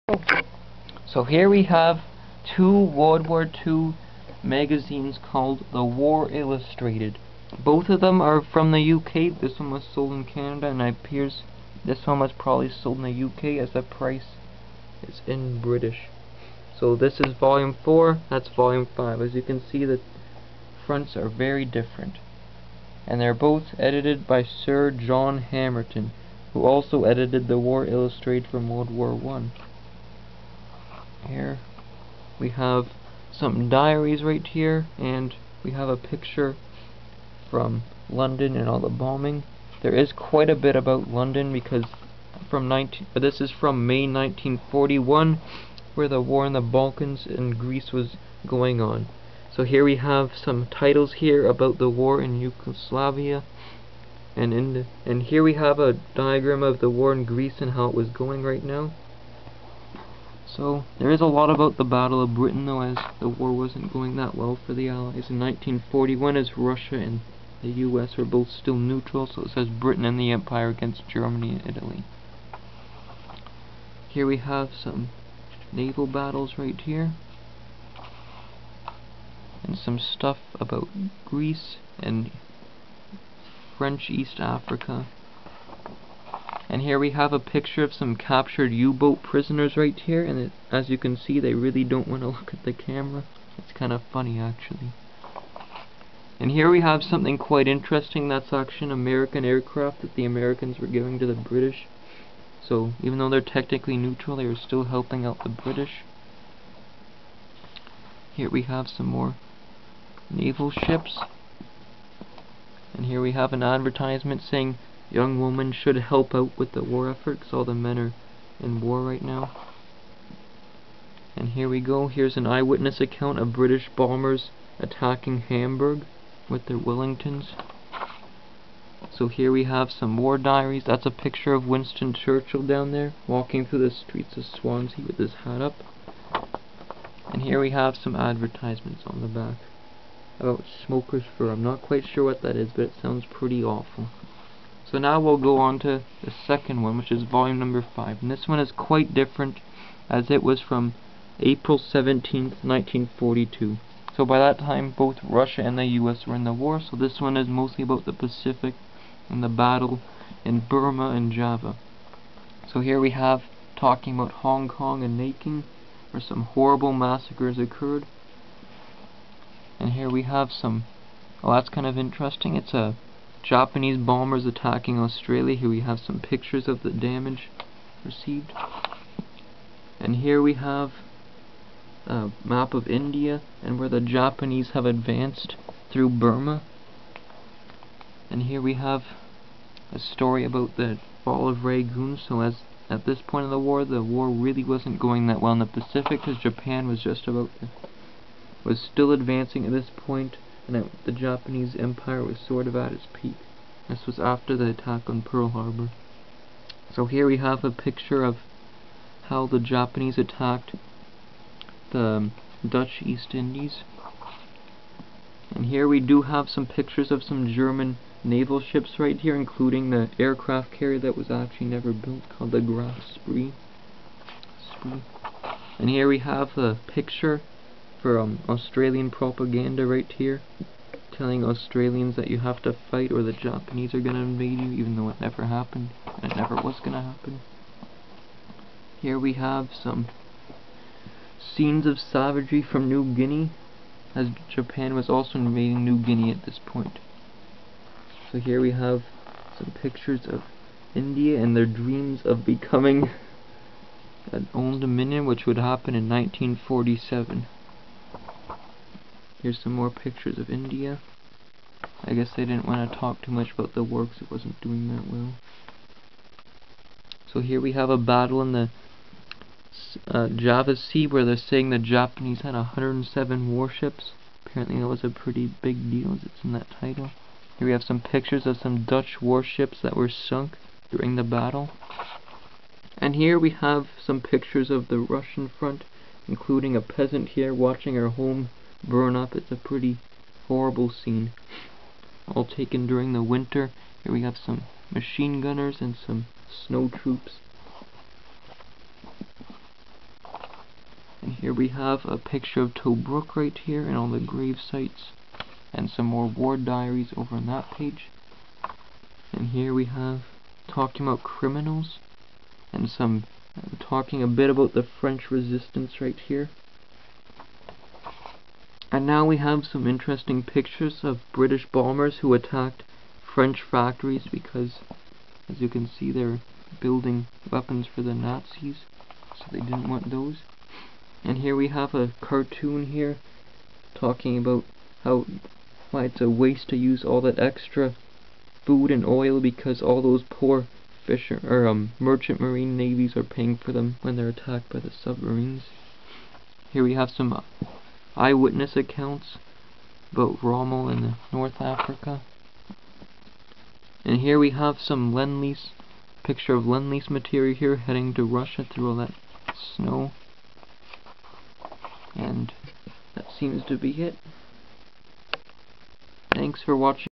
The War Illistrated WWII Magazines
Two WWII magazines from 1941 and 1942 made by Britain during WWII.